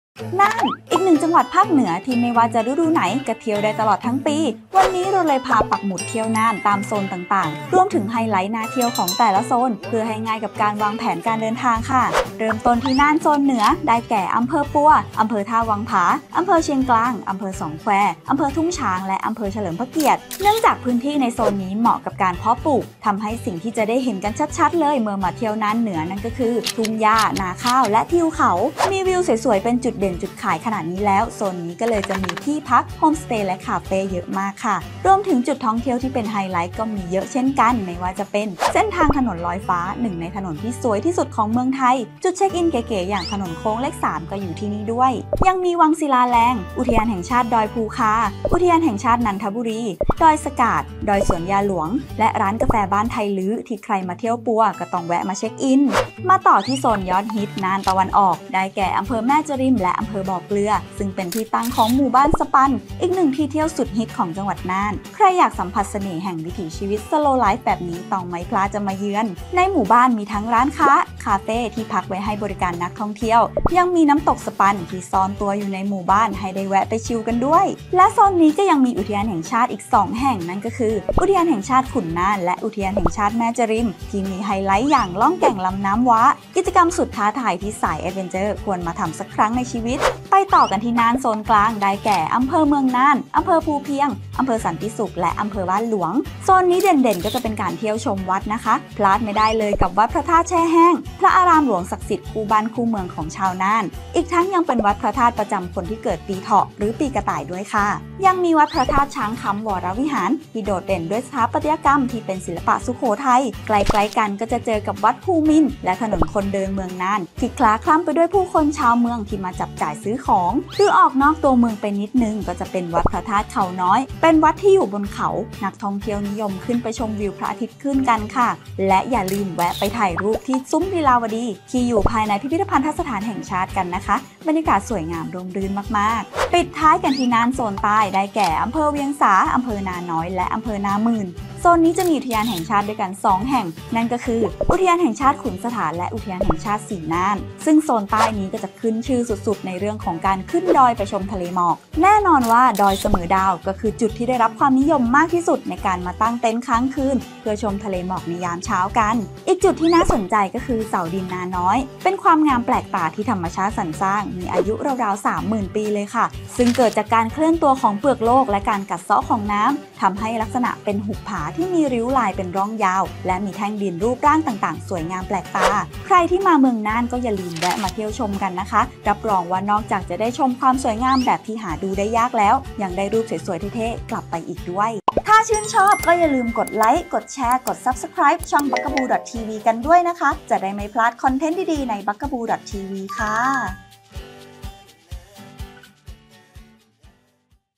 30 ที่เที่ยวน่าน ชมธรรมชาติสุดฟิน อินล้านนา 2567 | Bugaboo Travel | Ep.94
เที่ยวน่าน 2567 จะหน้าร้อน หน้าฝน หรือหน้าหนาว จังหวัดน่าน ก็เป็นอีกหนึ่งจังหวัดภาคเหนือ ที่มีสถานที่ท่องเที่ยวมากมาย เต็มไปด้วยวัฒนธรรมประเพณี และธรรมชาติที่สวยงาม วันนี้เราเลยมาพาปักหมุดเที่ยวน่าน กับ 30 ที่เที่ยวน่าน ชมธรรมชาติสุดฟิน อินล้านนา 2567